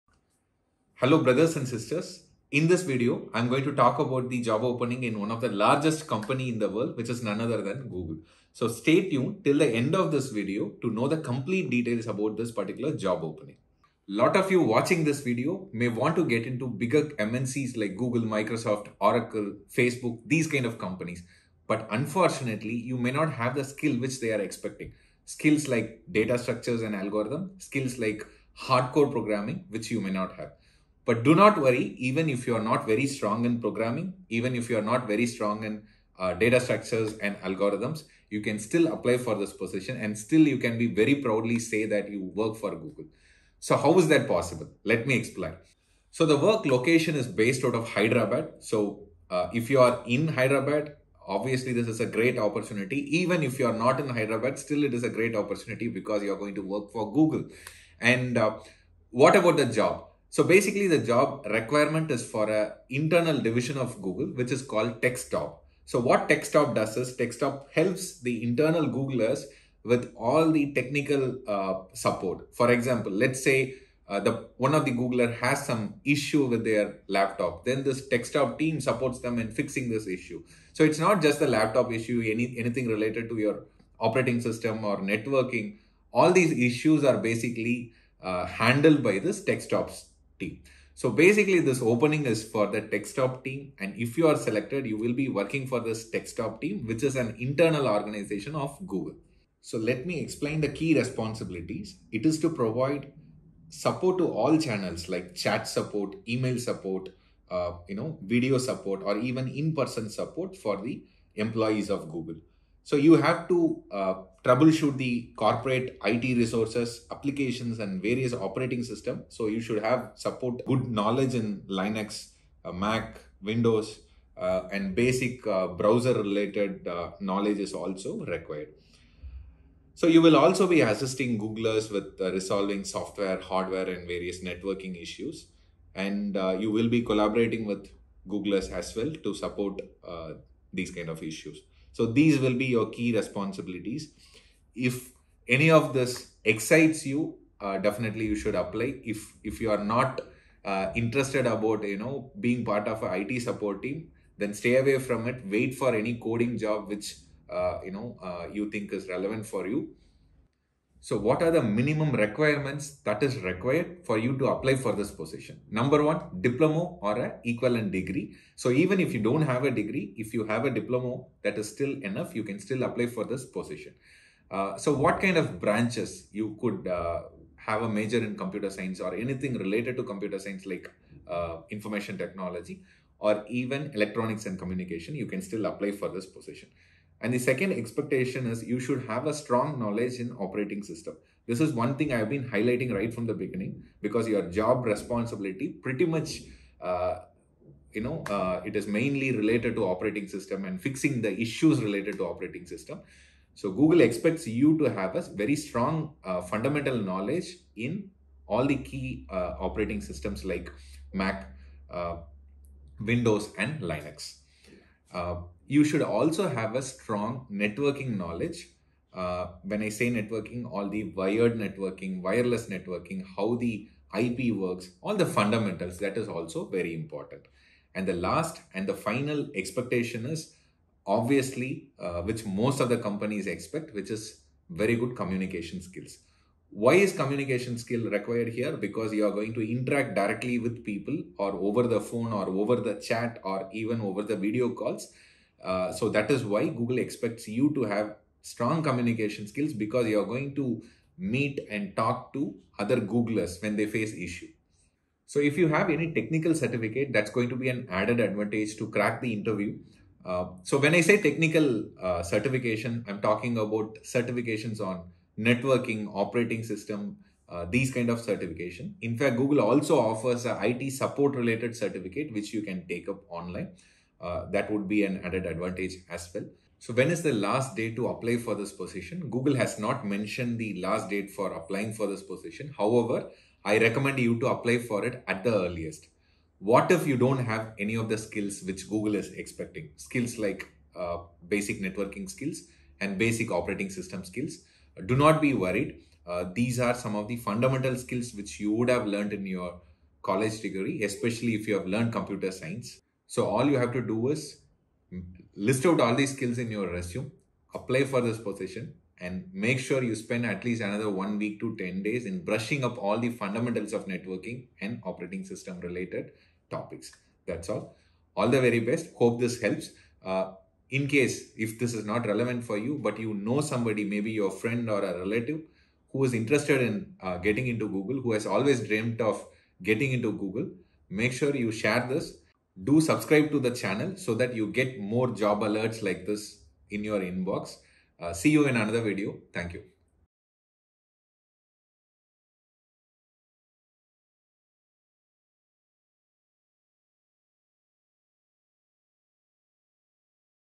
Google Hiring Freshers | Google Recruitment | IT Support Engineer | Hyderabad
Google hires IT Support Engineer in Hyderabad.

Still you are struggling to get an IT Job?
Learn at FITA Academy and get your Dream IT Job with 100% Placement Support.